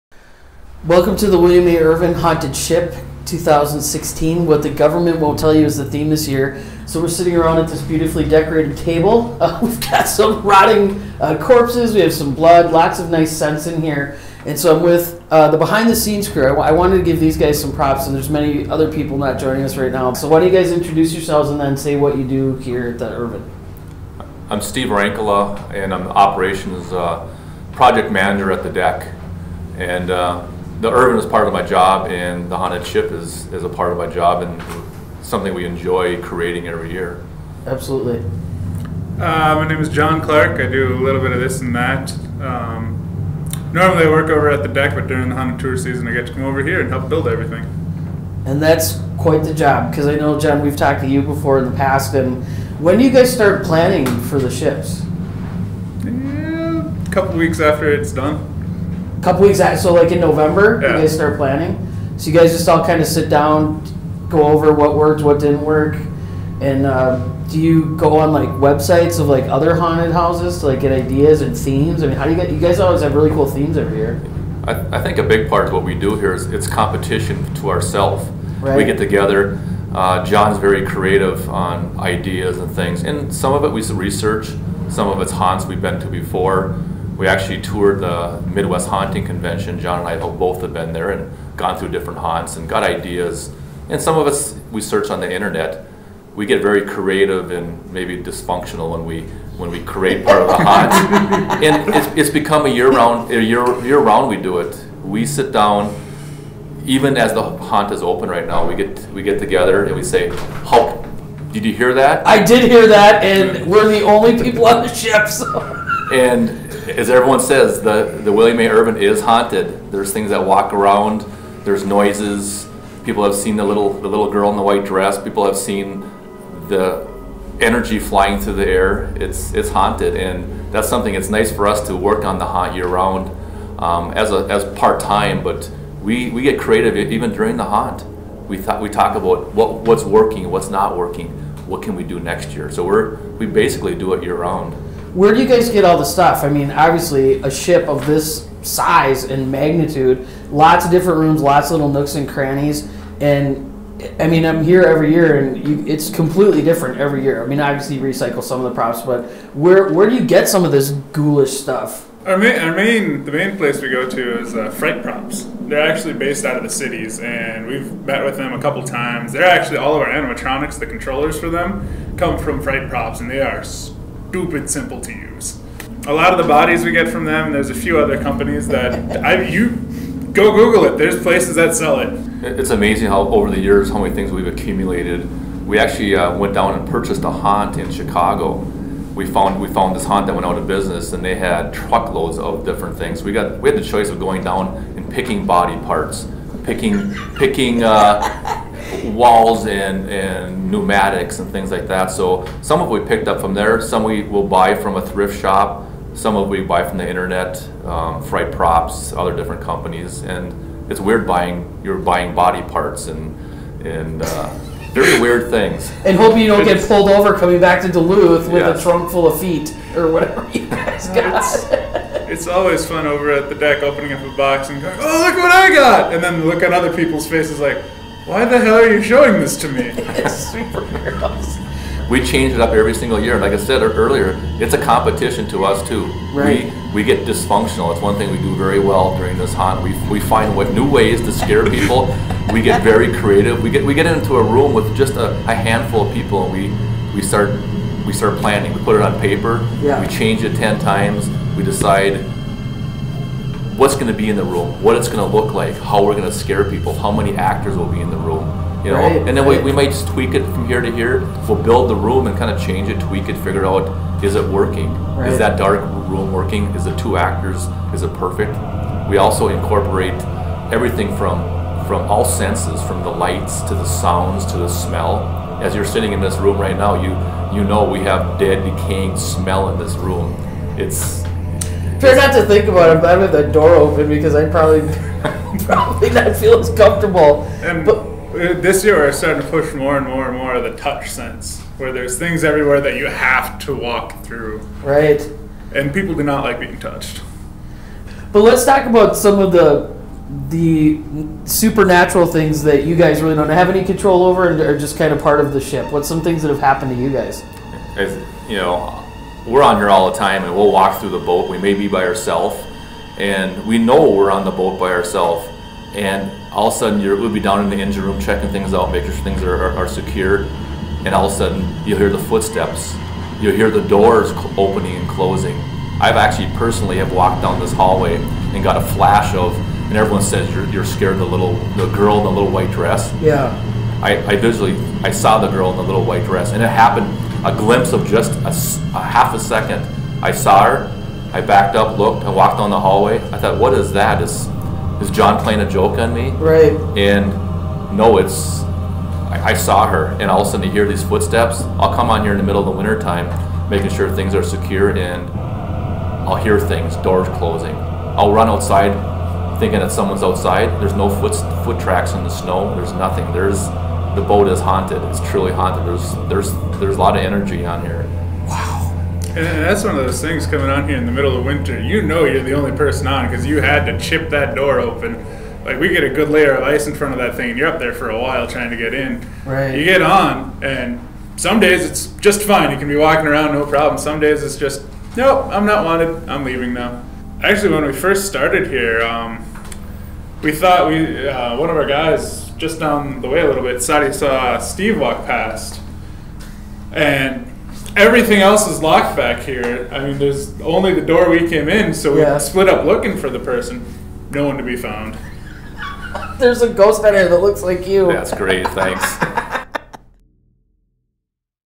Haunting the William A. Irvin Haunted Ship for the 2016 Halloween Season
Learn what it takes to decorate the William A. Irvin Haunted Ship in Duluth, getting it ready for this year's theme 'What the Government Won't Tell You', and hear true ghost stories from the crew that manage the ship year-round.